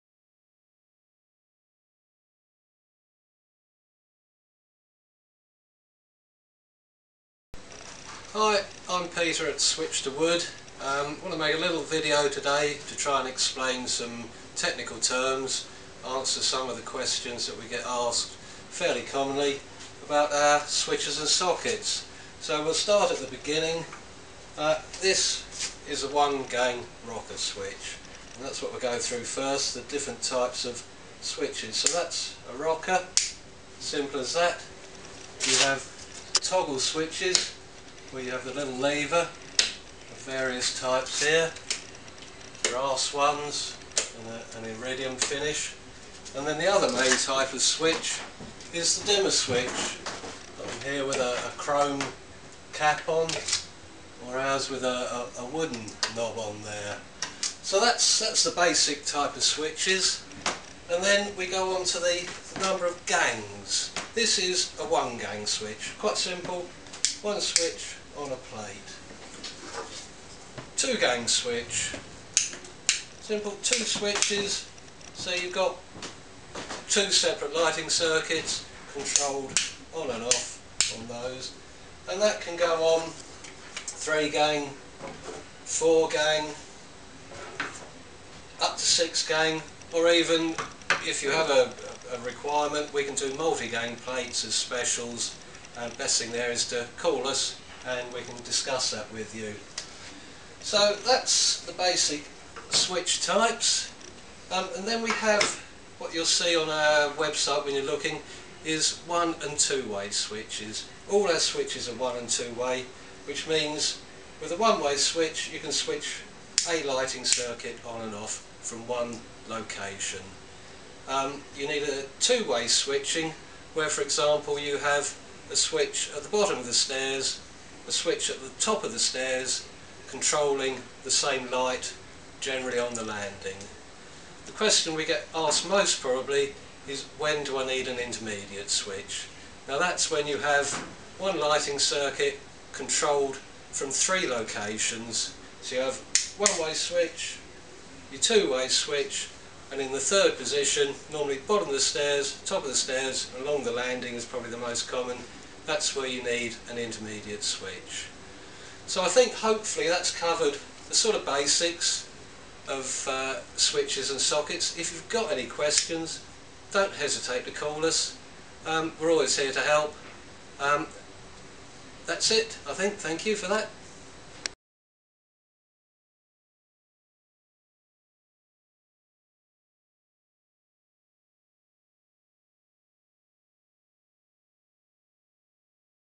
What is an Intermediate switch? What is 1 and 2 way and gangs explained!
Switch to Wood helps you understand what an Intermediate switch is and where to use it. The video and also explains other technical terms such as "1 and 2 way" and "gangs".